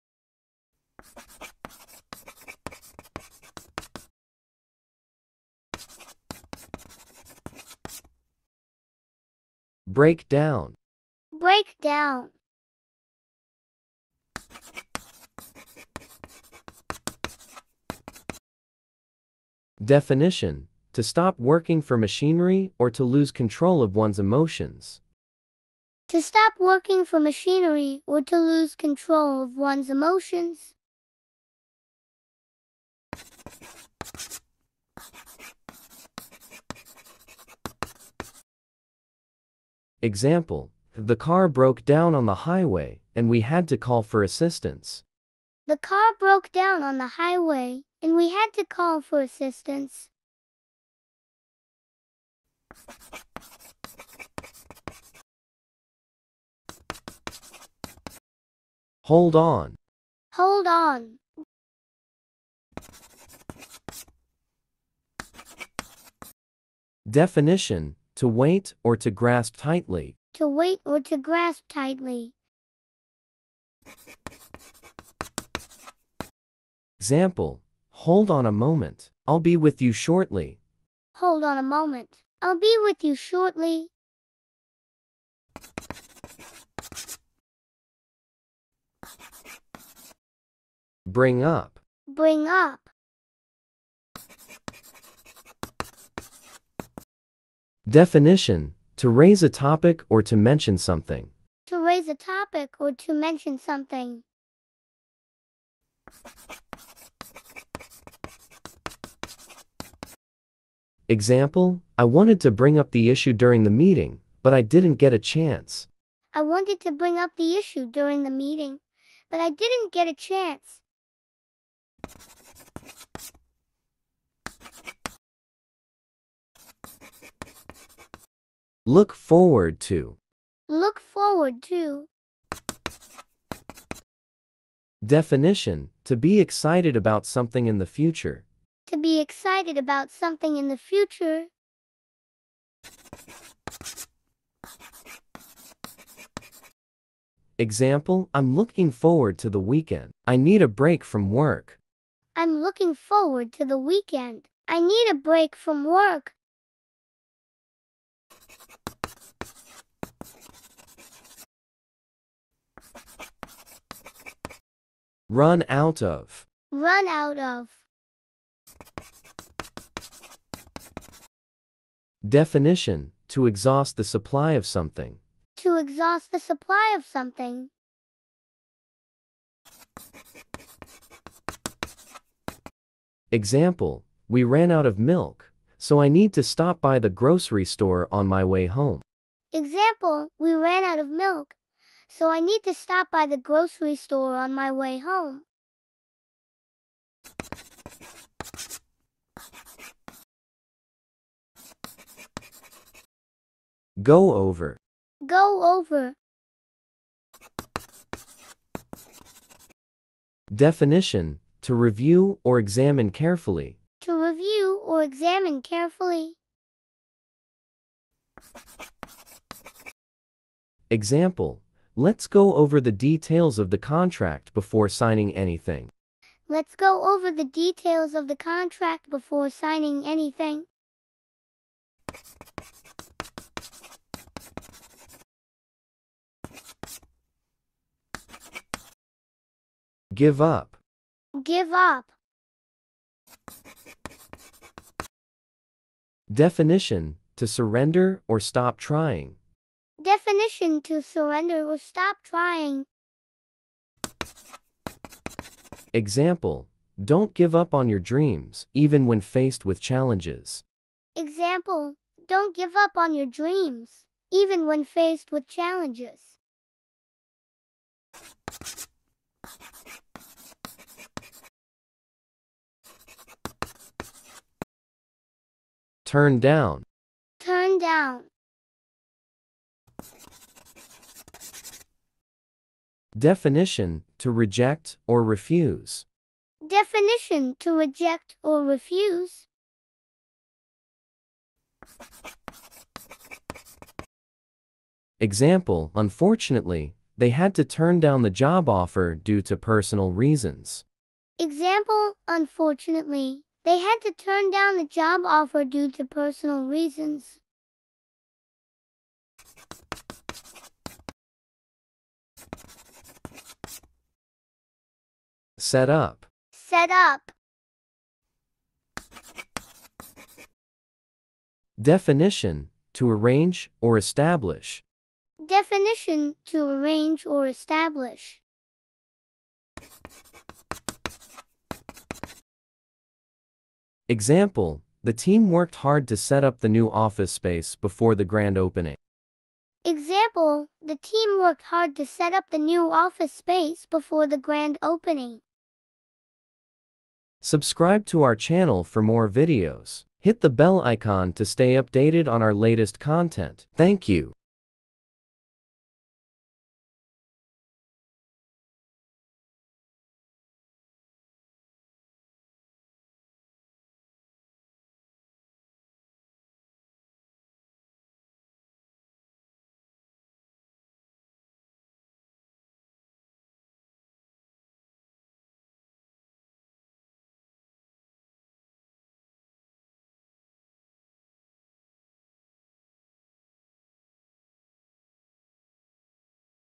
Vocabulary Phrasal Verbs l Phrasal Verbs with Definition and Sentences l Listen and Practice.
Vocabulary Phrasal Verbs l Phrasal Verbs with Sentences l Listen and Practice.

"Kindly show your support by liking, commenting, and sharing. Your engagement means a lot to us!

Phrasal verbs with sentences
Vocabulary Phrasal verbs.